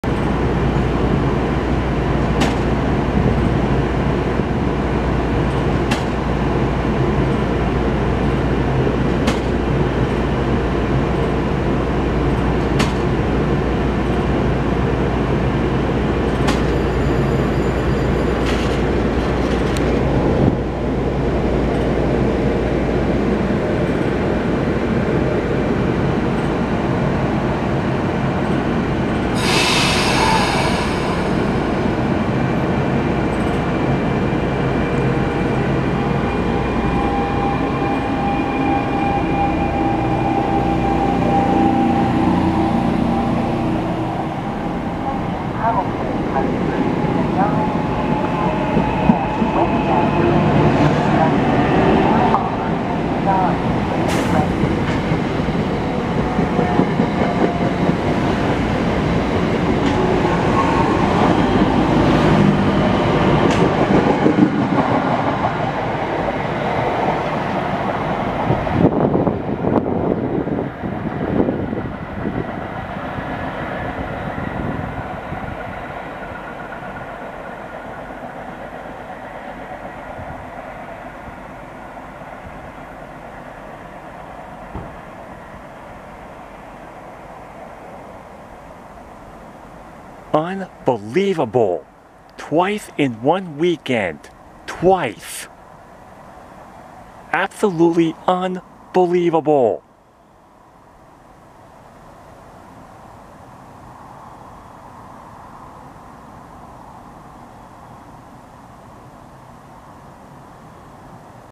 NJ Transit New York Bound 10-Car Train Of GE Arrow III EMU's Departing Hamilton
Here is an NJ Transit train of GE Arrow III EMU's operating on the Northeast Corridor line to New York departing from Hamilton Station. My reaction clearly tells it all since I was just stunned. It has never happened before.